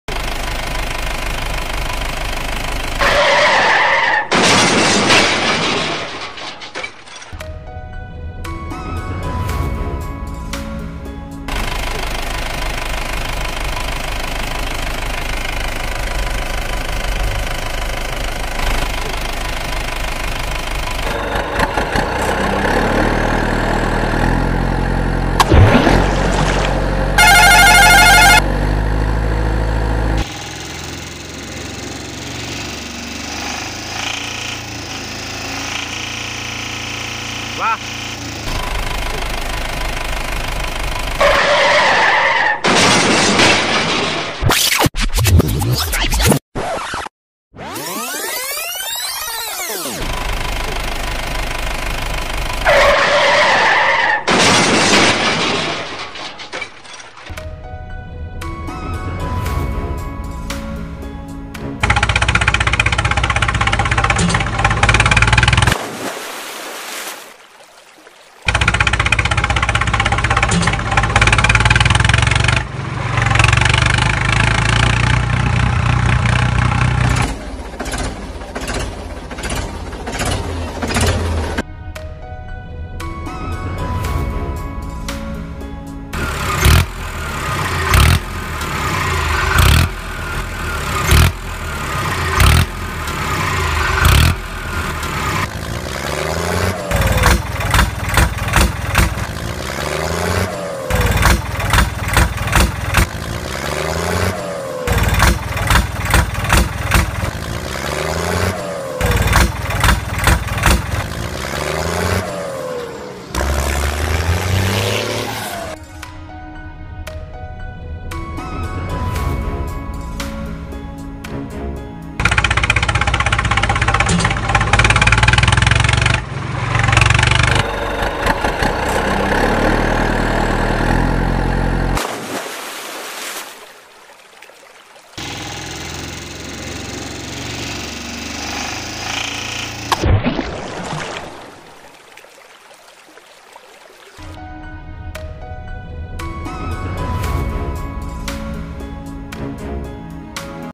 Mahindra Bolero Accident Highway Pit Pulling Out Mahindra Tractor ? Tata Pickup | Dump Truck | CSToy
video Tital  :-  Mahindra Bolero Accident Highway Pit Pulling Out Mahindra Tractor ? Tata Pickup | Dump Truck | CSToy